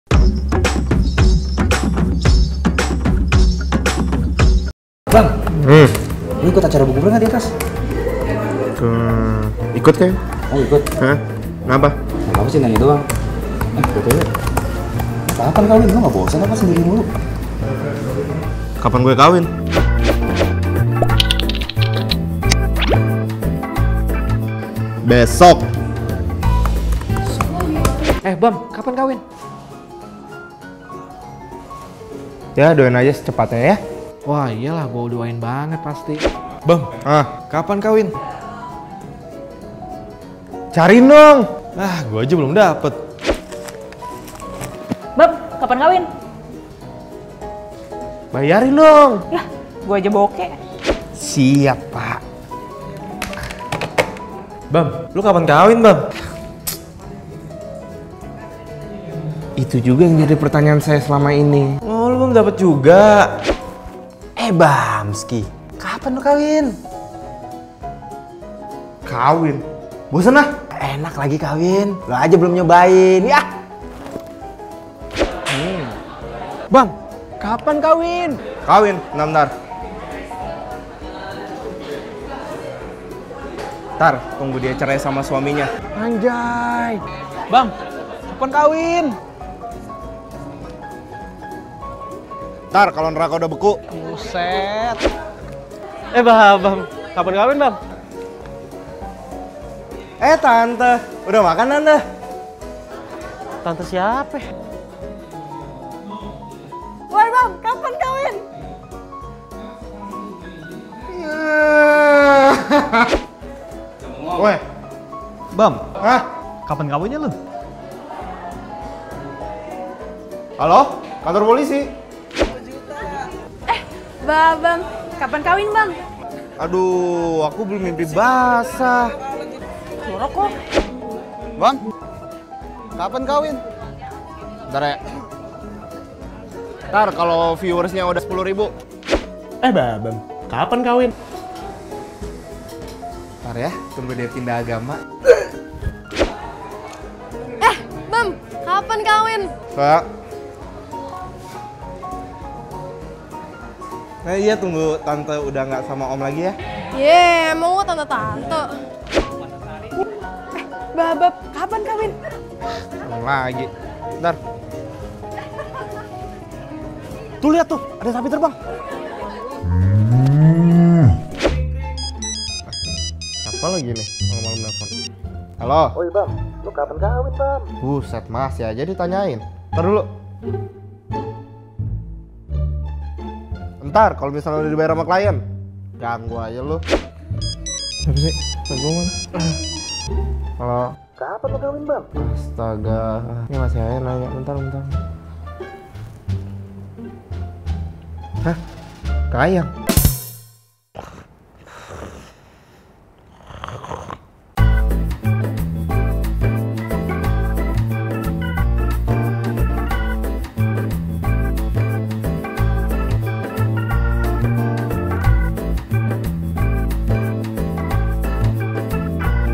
Cara Ngeles Kalau Ditanya "Kapan Kawin?" (kapanguekawin.com)
Ketika kamu udah survive ditanyain "kapan lulus?" beberapa tahun lalu. Kamu belum bisa senang begitu aja, karena masih ada kalimat "kapan kamu kawin?". Sebuah pertanyaan sakral yang bawaannya pengen kabur dari obrolan ini. Tenang! MBDC punya solusinya, yuk buka kapanguekawin.com dan berbahagialah!

CREDIT TITLE:
Producer/Director: @ranmamuse
DOP: @dwariya
Soundman: @ranmamuse
Editor: @anggancet @ranmamuse
Motion: @heykila @ranmamuse